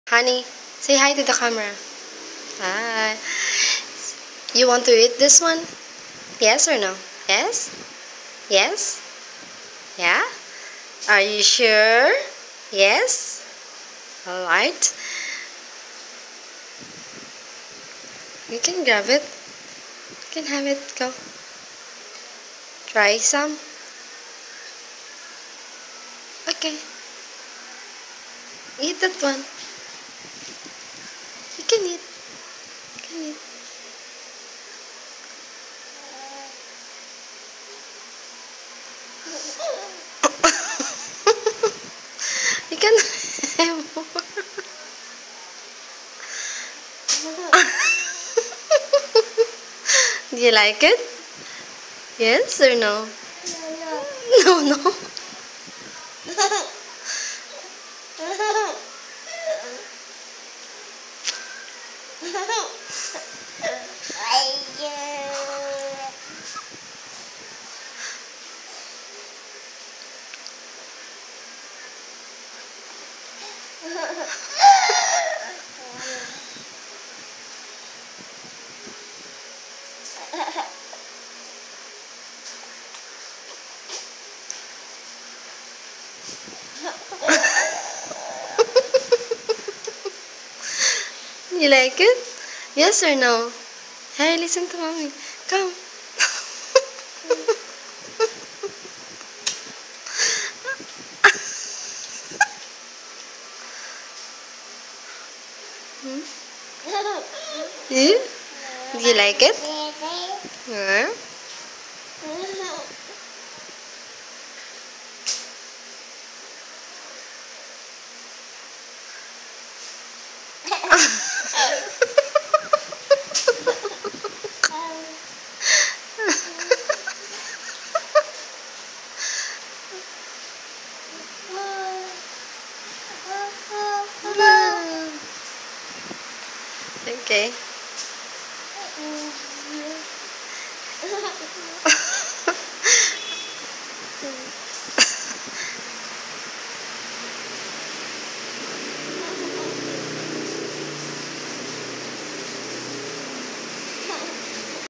Baby Le'an eating lemon :)
See his reaction when he start to eat lemon :)